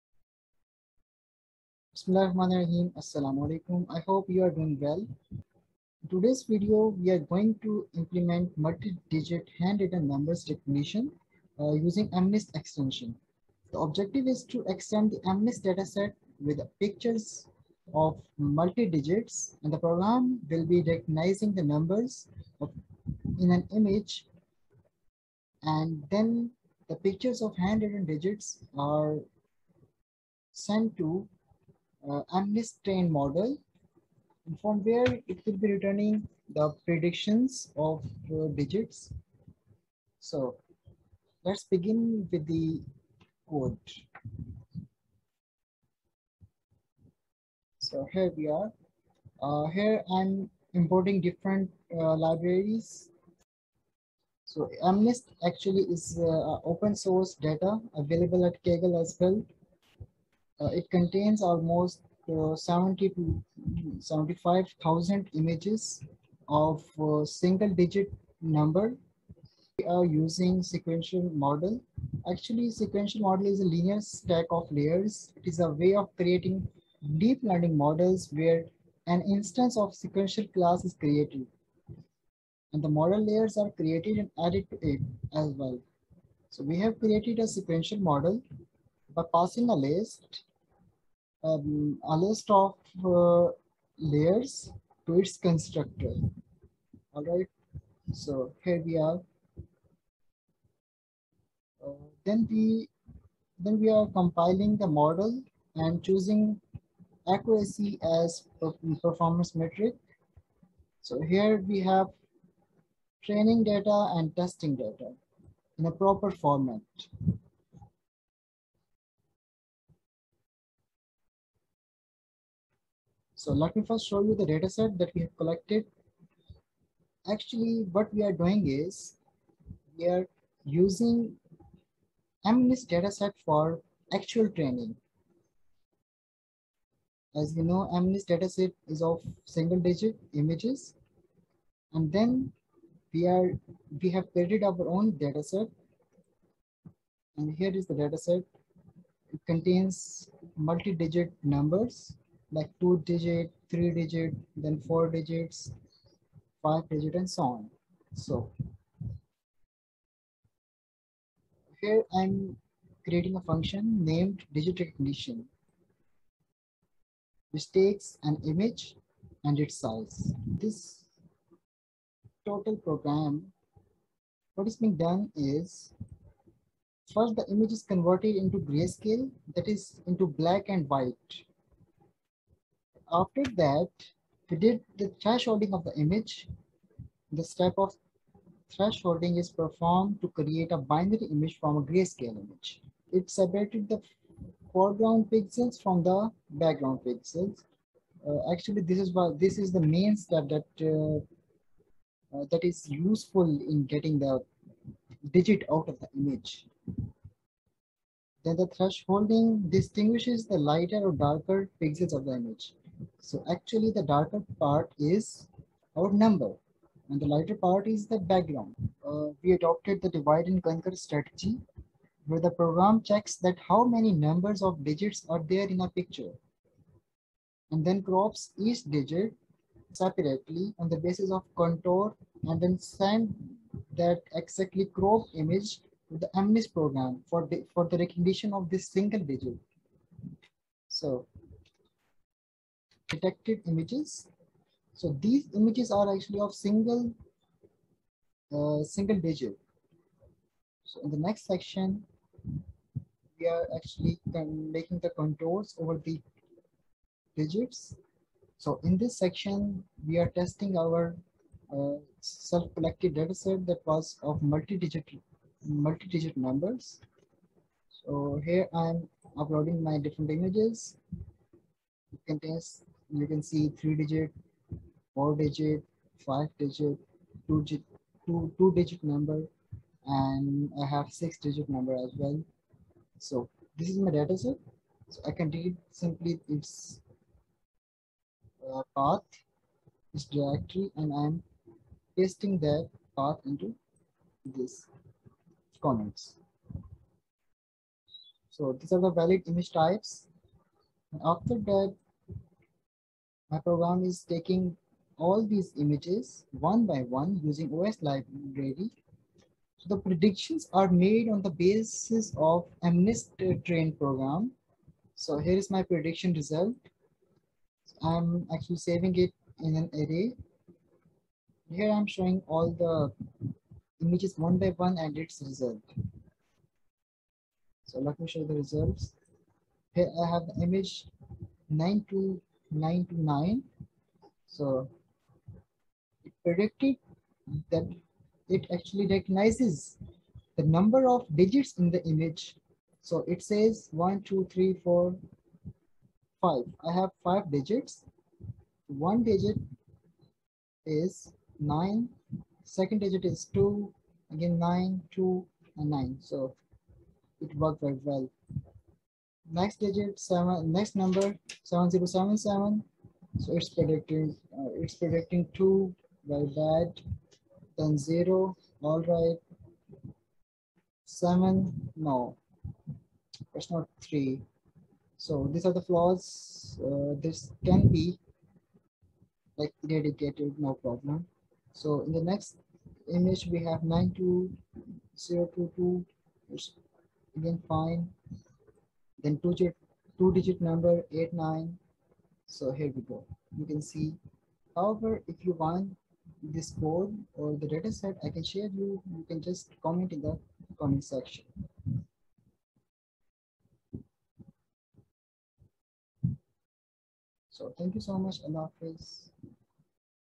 Multi-Digit Handwritten Number Classification | Dataset| Python Code
Handwritten Multi-Digit Number Recognition
In this program the image is converted into grey scale that is into black
and white.
After converting into gray-scale, we did the thresholding of the image. The step of thresholding is performed to create a binary image from a gray-scale image. It separated the foreground pixels from background pixels of image. Thresholding distinguishes the lighter or darker pixels of the image. And we set the value of thresholding such that it gives us the darker part of the image.